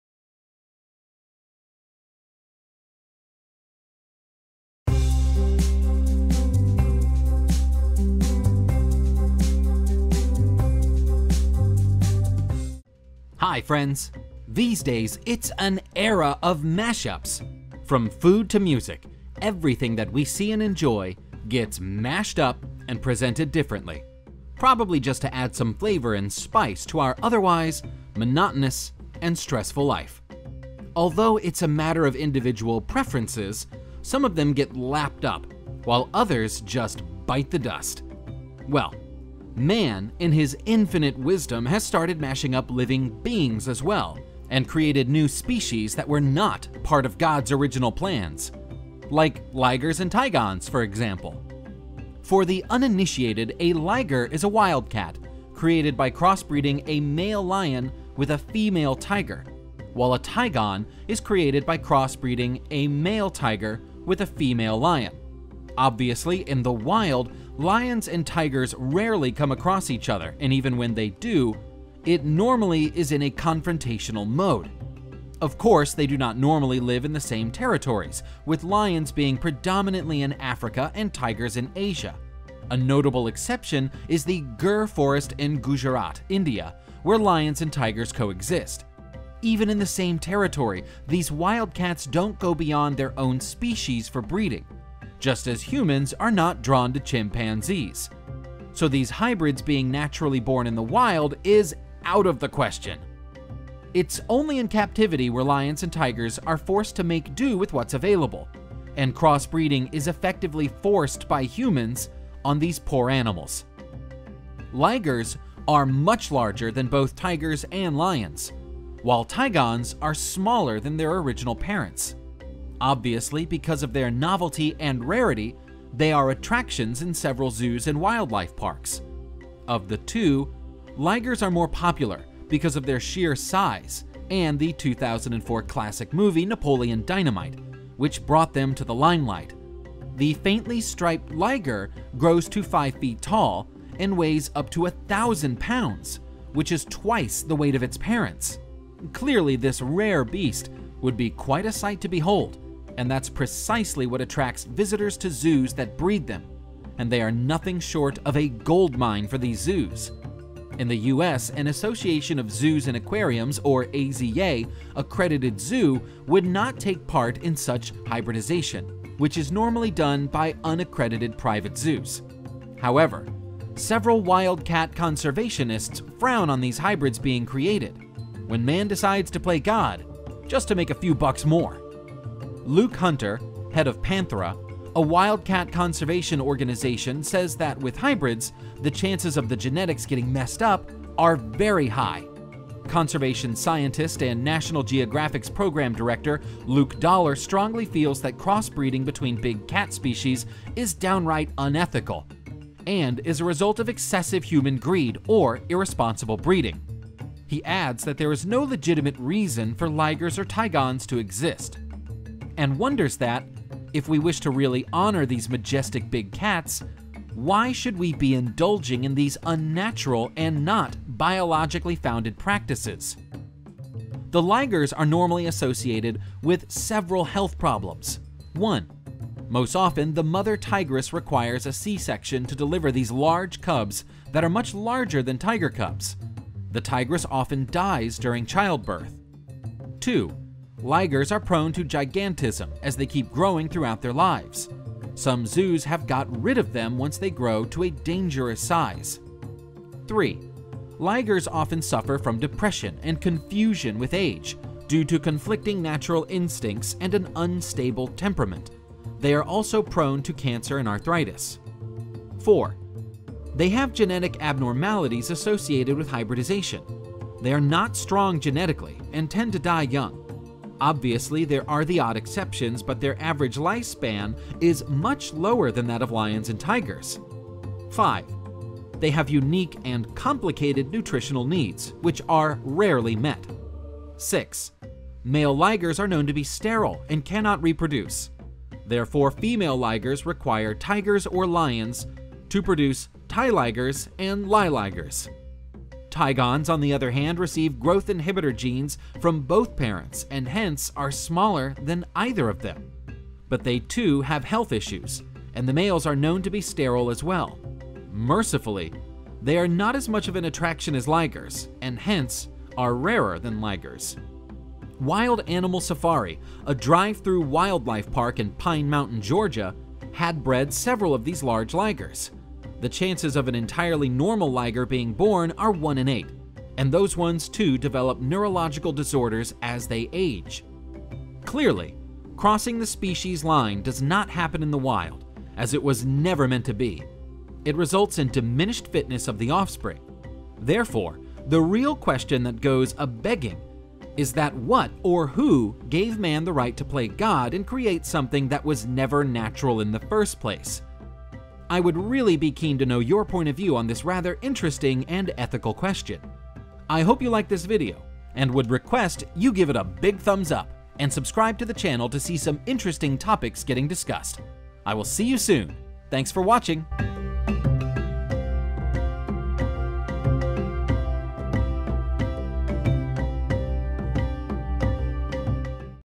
IS IT ETHICAL TO BREED GIANT HYBRID LIGERS AND TIGONS?
These days, its an era of mash-ups! From food to music, everything that we see and enjoy, gets mashed up and presented differently…. probably just to add some flavour and spice to our otherwise monotonous and stressful life! Although its a matter of individual preferences, some of them get lapped up, while others just bite the dust! Well, man in his infinite wisdom has started mashing up living beings as well, and created new species that were not part of God’s original plans….like ligers and tigons for example!